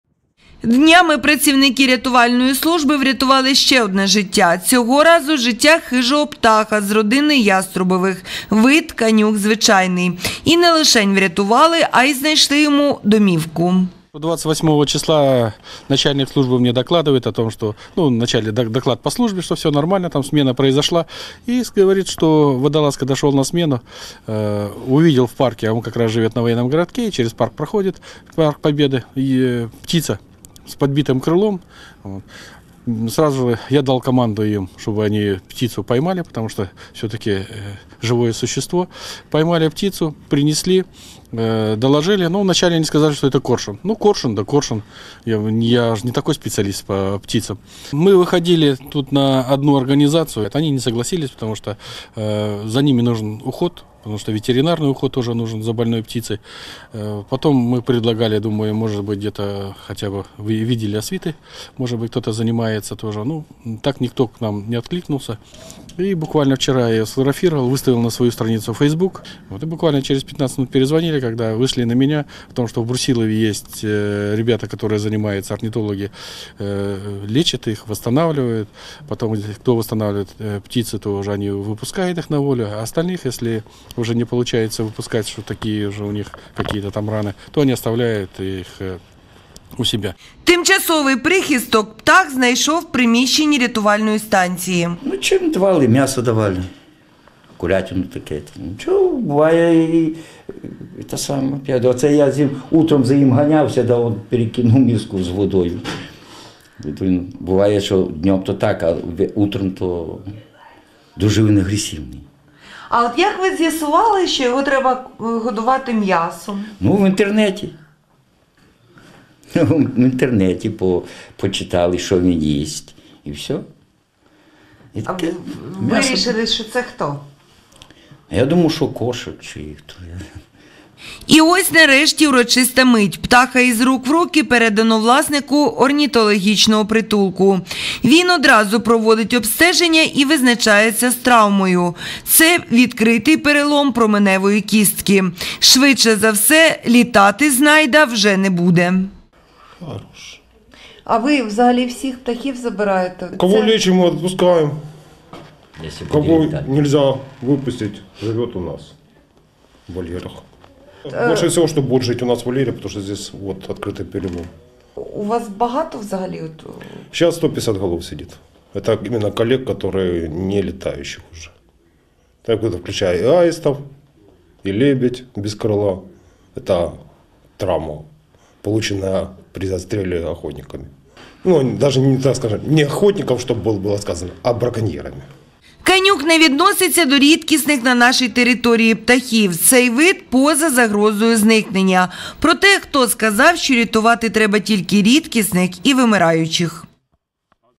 KorostenTV_11-01-19_Врятували хижого птаха
Зима – важкий період для диких тварин. Особливо травмованих. І тут дуже важливо зустріти людину чи людей, які захочуть і зможуть допомогти. Хижий птах з переламаним крилом був приречений на загибель, проте вижив і знайшов собі дім.